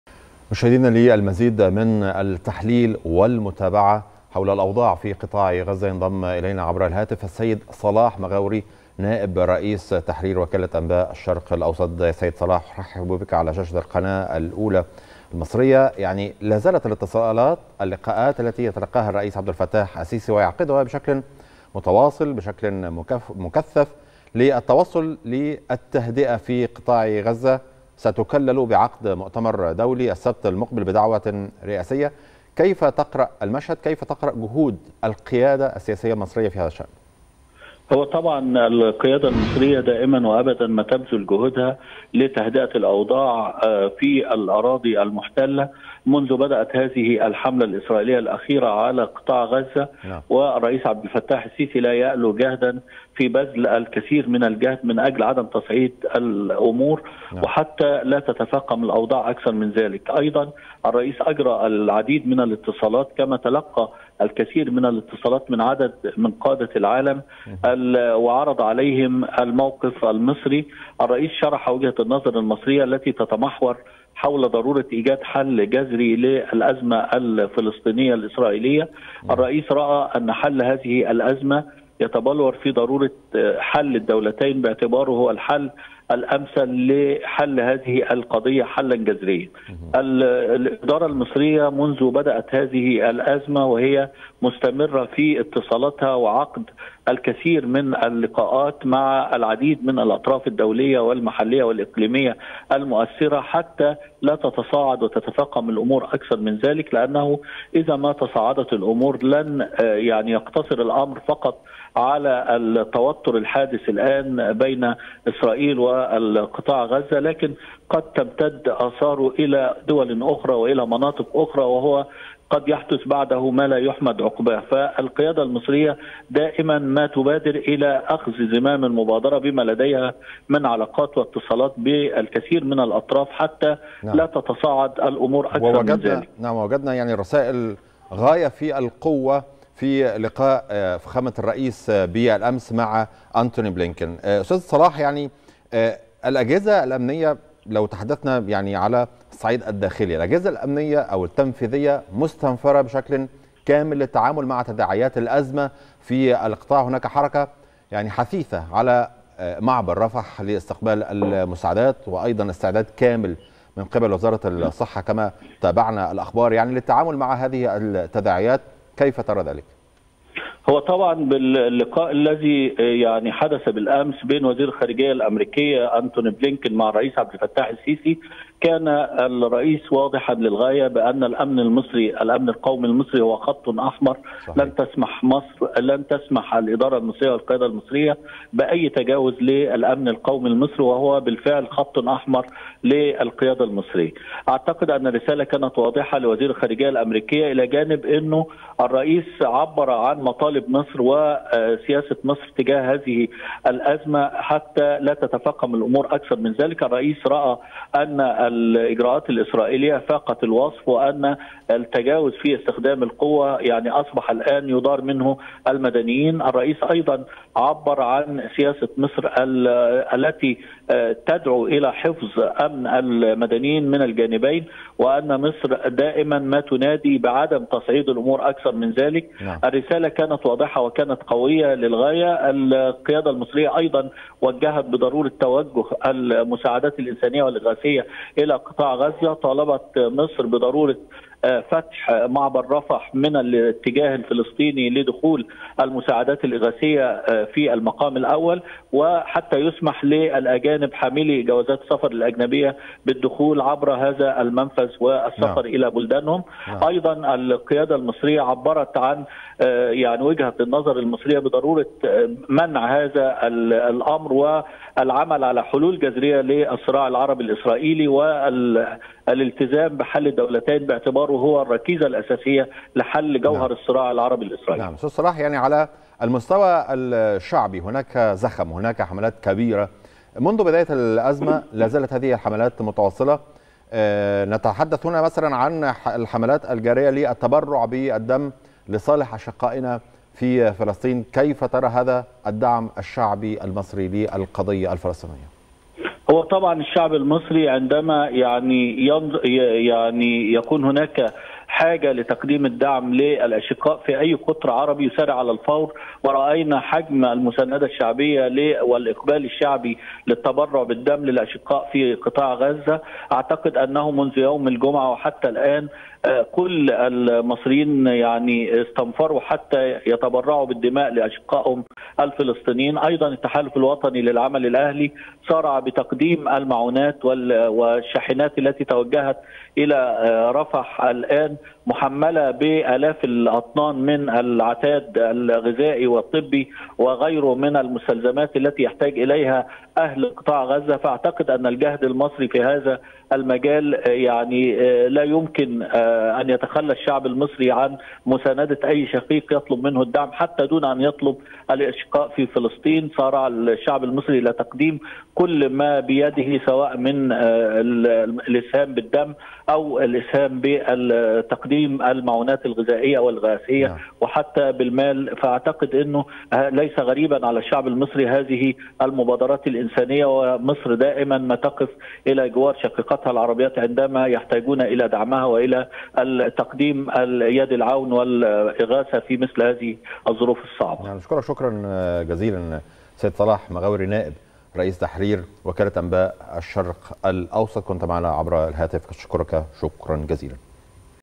توجيهات رئاسية لمنظومة الصحة يوضحها صلاح مغاوري نائب رئيس تحرير وكالة أنباء الشروق الأوسط
توجيهات رئاسية لمنظومة الصحة للتعامل مع أي طوارئ طبية يوضحها صلاح مغاوري نائب رئيس تحرير وكالة أنباء الشروق الأوسط